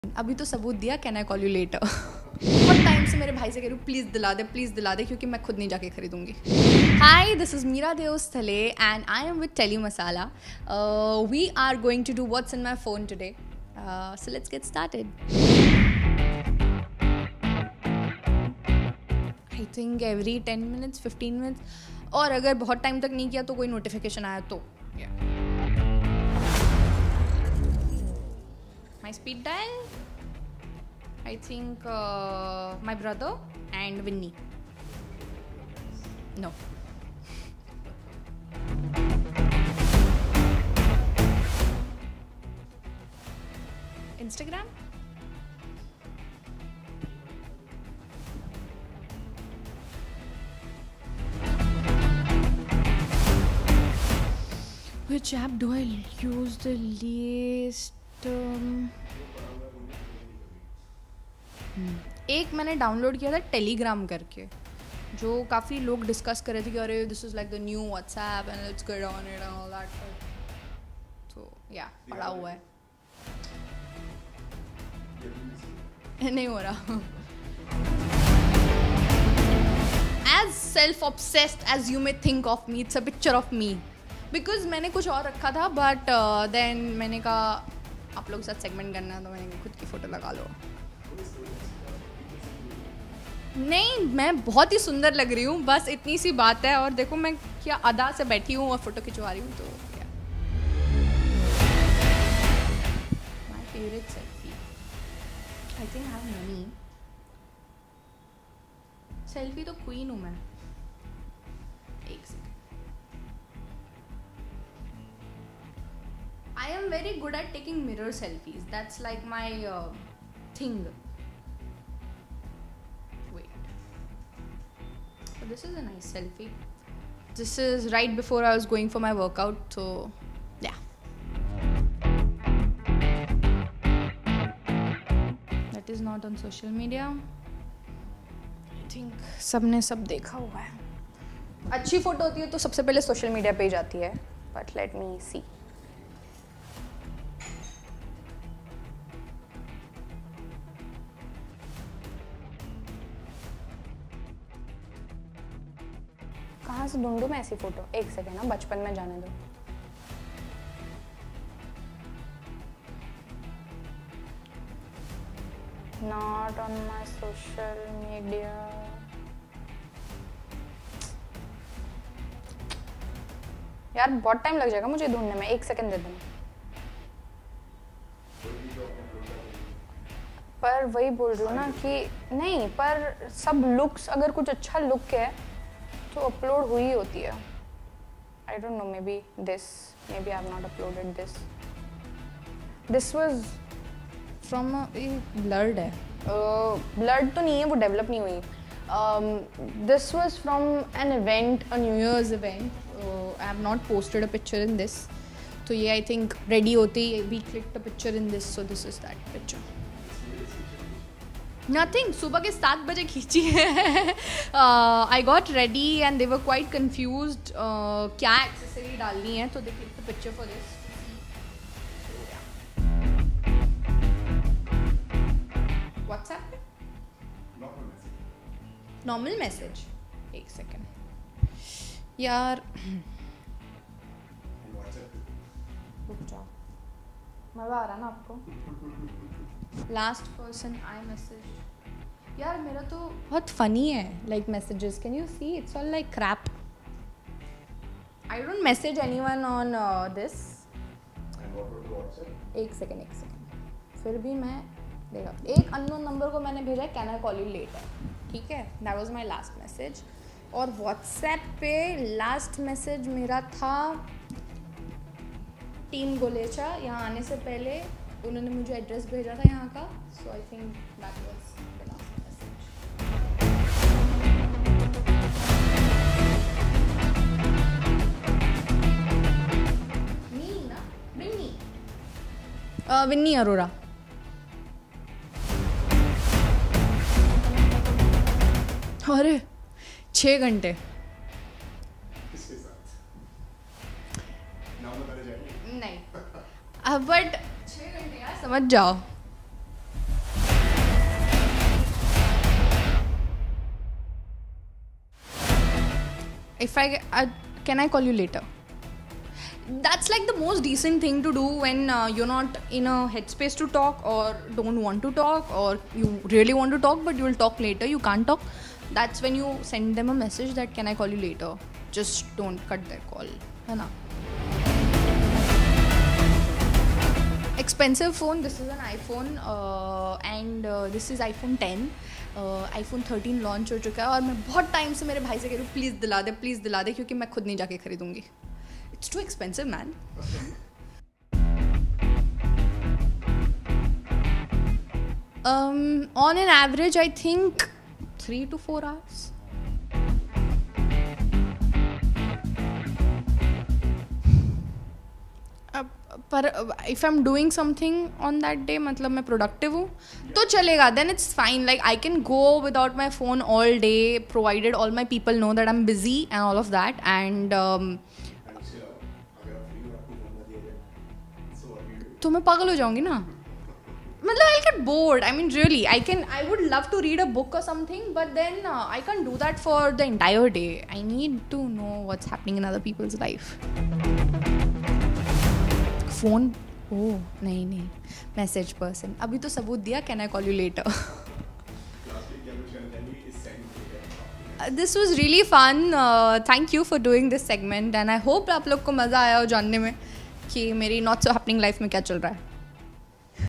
Meera Deosthale Ft. What's In My Phone | Phone Secret REVEALED | Exclusive
Actress MEERA DEOSTHALE shares her phone secrets exclusively with Telly Masala. Watch the video to know more.

Reporter: Faizan Syed
Editor: Rahul Gamre
Cameraman: Deepak Prajapati